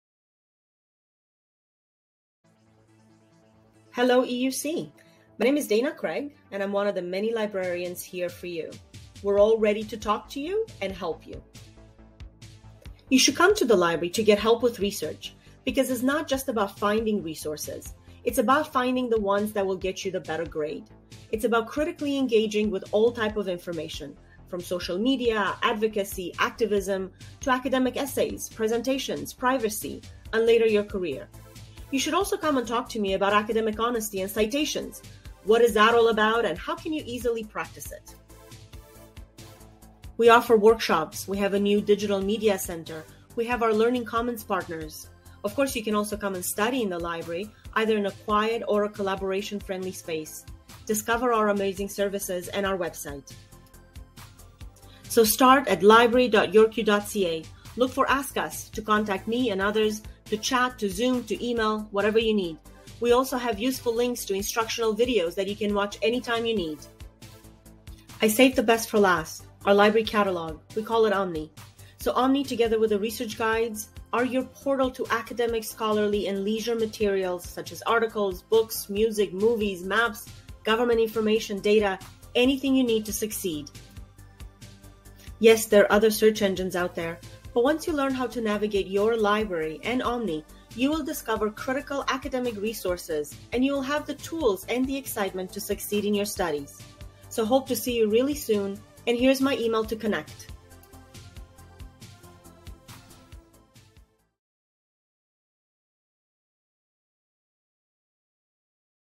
Hello to EUC Changemakers from your Librarian, Dana!
A special introduction video from the Faculty of Environmental & Urban Change's very own Librarian, Dana Craig! Learn about how Dana can support your research with the incredible library services available at York University.